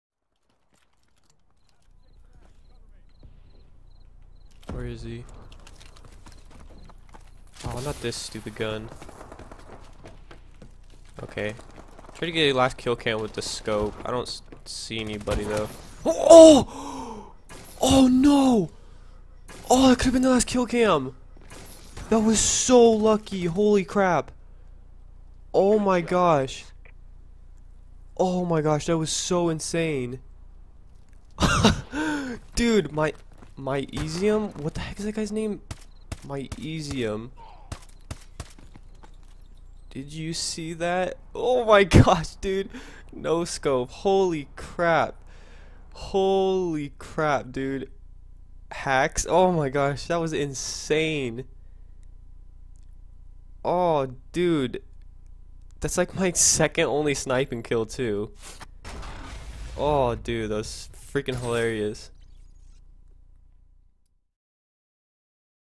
FAZE NATE
LOL wut!? I can't hit anything with the scope and then hit that on accident...